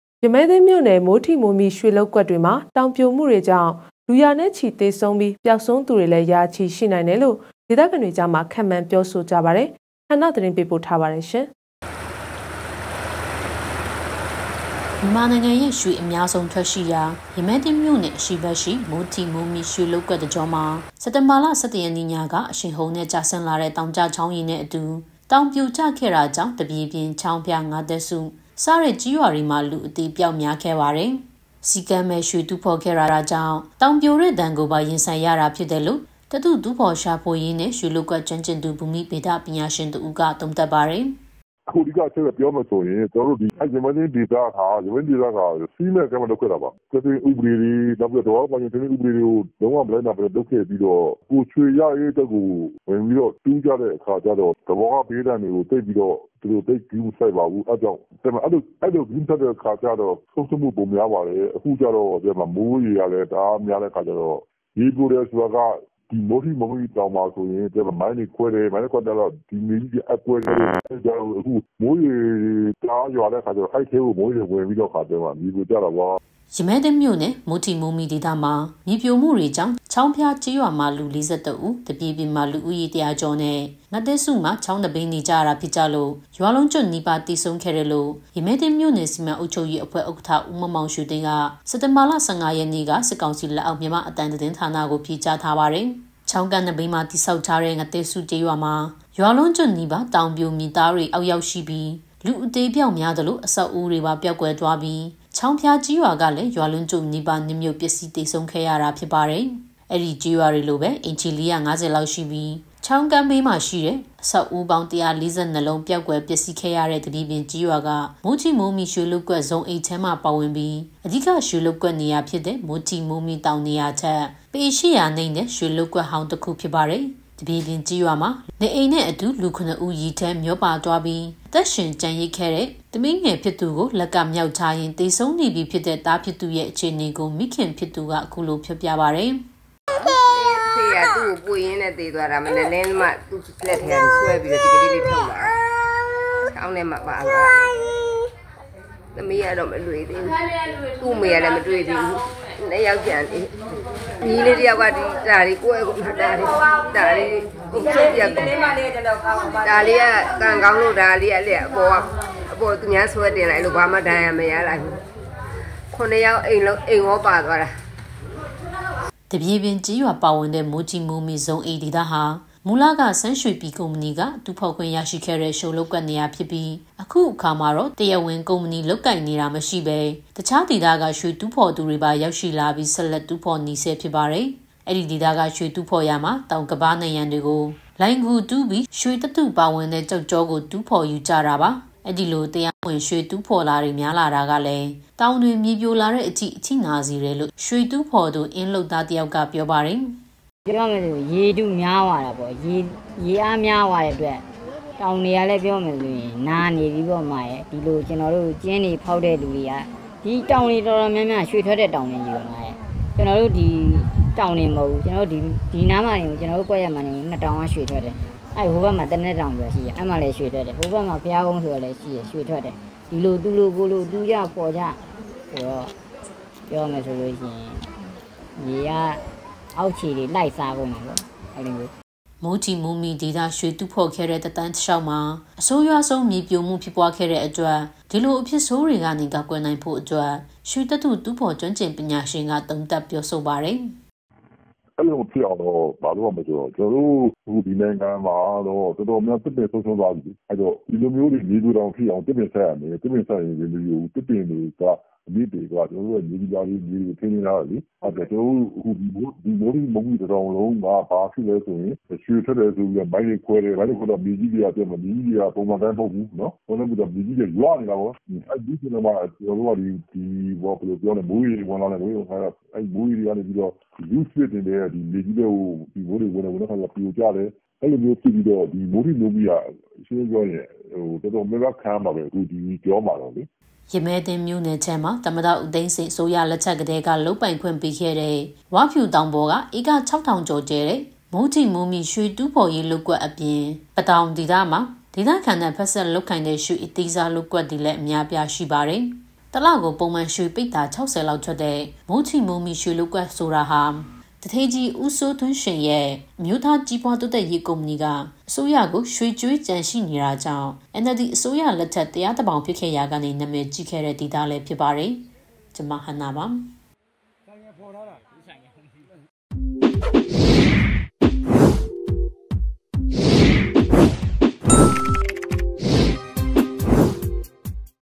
မိုးထိမိုးမိရွှေလုပ်ကွက်တွေမှာ ရေဘေးကြောင့် လူရာချီသေဆုံး၊ ပျောက်ဆုံး- DVB News
ရမည်းသင်းမြို့နယ် မိုးထိမိုးမိရွှေလုပ်ကွက်တွေမှာ တောင်ပြိုမှုကြောင့် လူရာနဲ့ချီ သေဆုံးပြီး ပျောက်ဆုံးသူတွေလည်း ရာချီရှိနိုင်တယ်လို့ ဒေသခံတွေကြားမှာ ခန့်မှန်းထားပါတယ်။